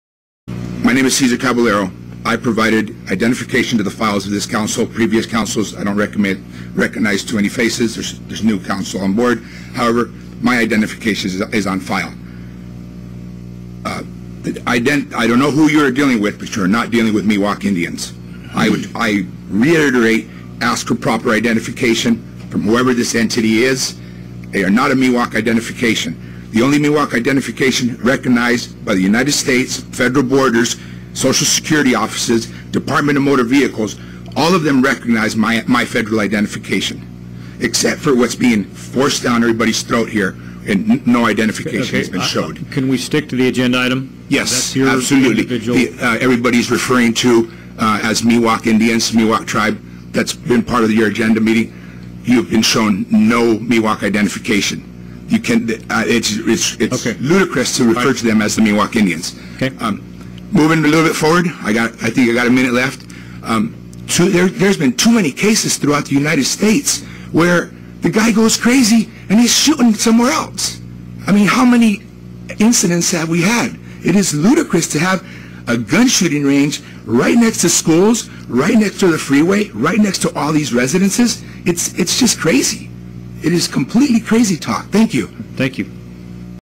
Testimony that Shingle Springs Casino Tribe are not Miwok
At the 2-24-15 El Dorado County Board of Supervisors meeting, Cesar Caballero testifies that the Shingle Springs Band of Miwok Indians have not presented evidence to show that they are really Miwok Indians.

Video courtesy of El Dorado County